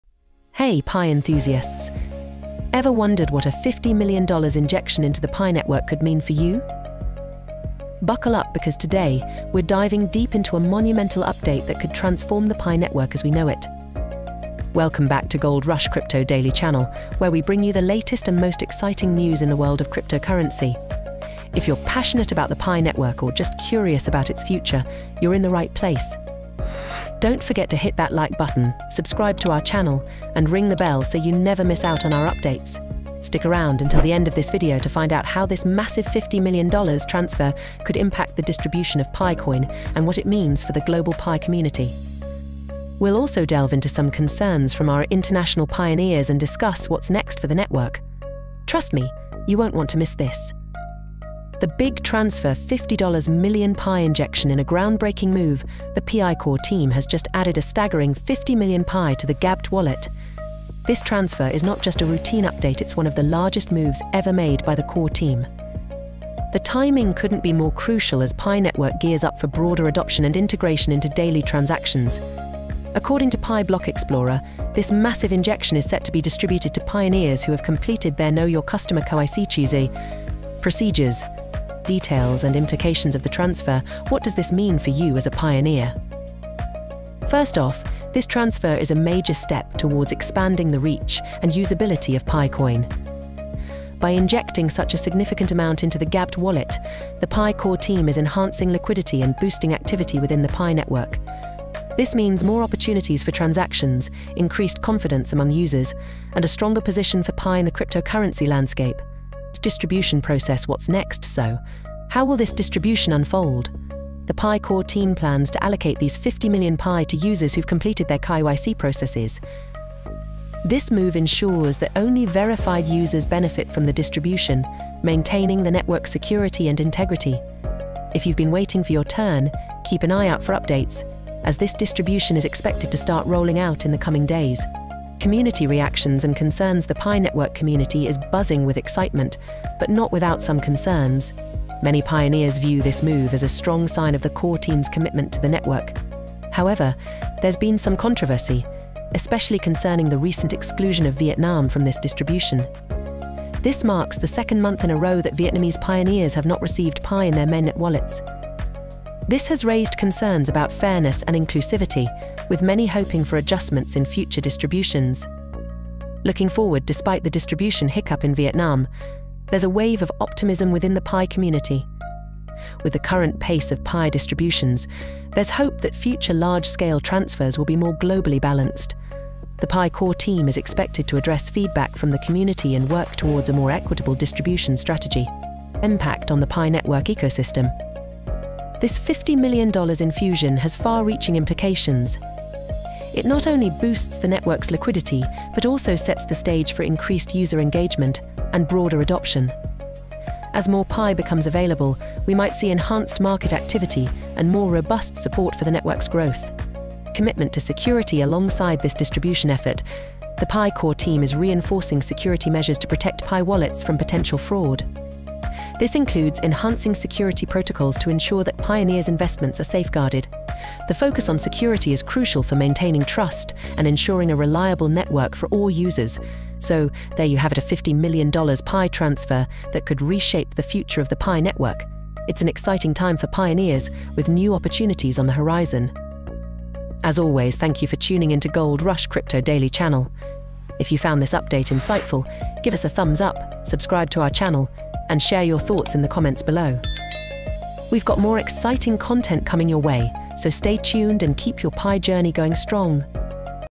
Pi Core Team's $50M Pi Transfer: What This Means for Global Distribution and Community Impact
Discover the latest groundbreaking move from the Pi Core Team as they inject 50 million Pi into the GABT wallet! This substantial transfer marks a pivotal moment for the Pi Network, setting the stage for broader distribution and heightened global engagement. Dive into the details of this significant development, the implications for the Pi community, and the ongoing discussions surrounding fairness in distribution. Don’t miss out—watch now to understand how this move could reshape the future of the Pi Network and what it means for Pioneers worldwide!